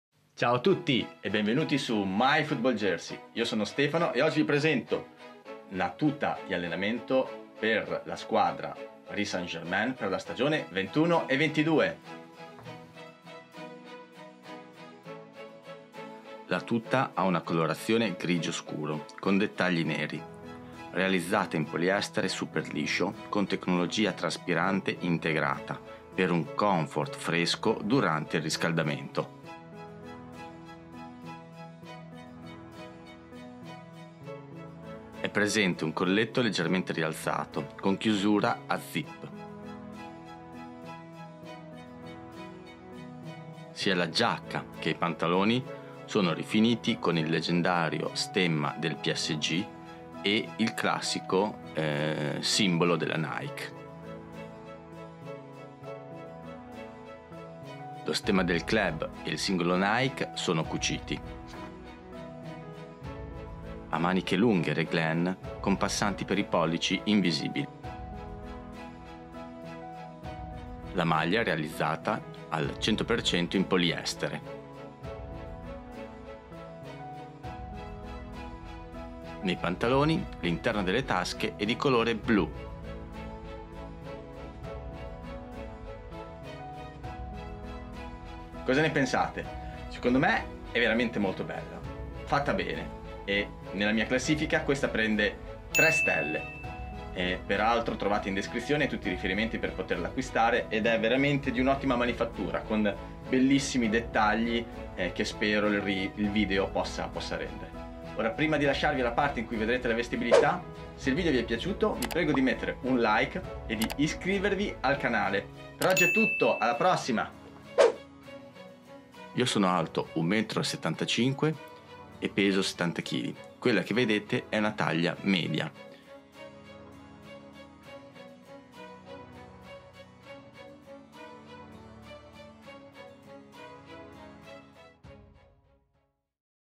SPtkit 21/22 PSG Tracksuit Grey (JJSport) Unboxing Review
Discover the 21-22 PSG Gray Half Pull Tracksuit

ATTENTION
Waiting for the spsets.com site to be restored I suggest you visit jjsport24.com

🕘 Timecodes
00:00 Overview
00:56 Details View
01:28 Ranking
01:57 Dimensions & Try On